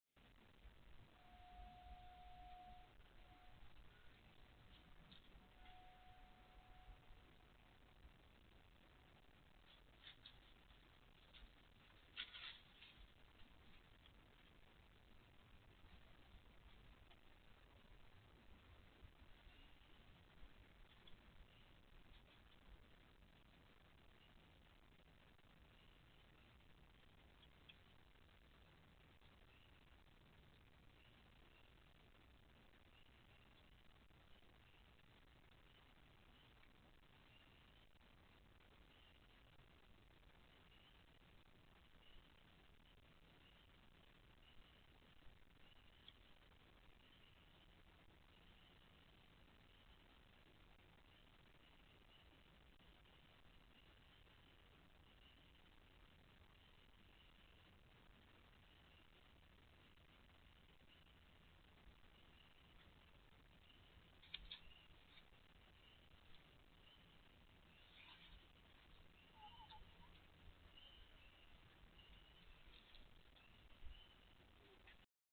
fox moving kit to new den
This fox has 6 kits in this den. A few hours before this clip was recorded a coyote showed up several times trying to get into the den.
By sunrise she had carried each kit to a new den.